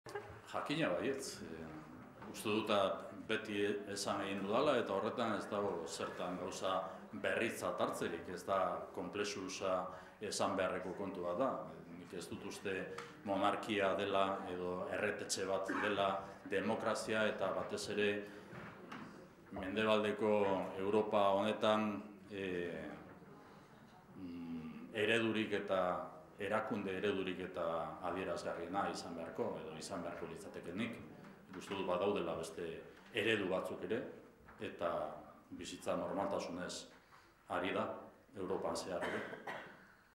Urkullu: 'Ez dut uste monarkia demokraziaren eredu denik'
Iñigo Urkullu lehendakaria Errepublikaren alde agertu da gaur goizean egin duen agerraldian. Europako beste demokrazia batzuk hartu ditu eredutzat.